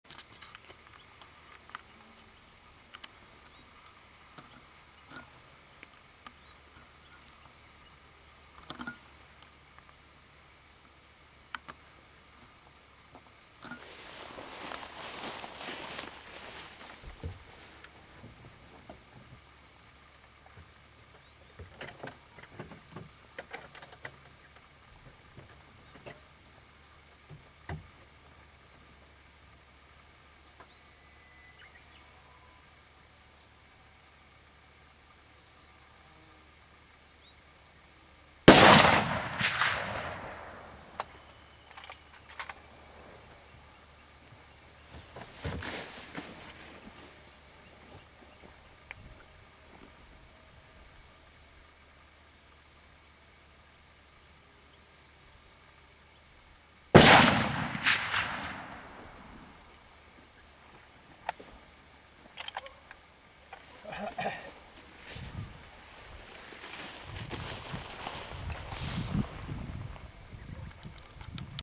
夏のエゾシカ駆除、倒れたのは、え～～後ろの大きい方かい！
昔、駆除が出来てた当時のビデオです！

確か小さいオスを狙って撃ったつもりでしたが、何故か後ろの大きなオスに命中してしまった、あ～～回収するのに少しでも軽いほうが良かったのに！
いつもの癖かな、無意識で大きなオスを狙ってしまったかも？！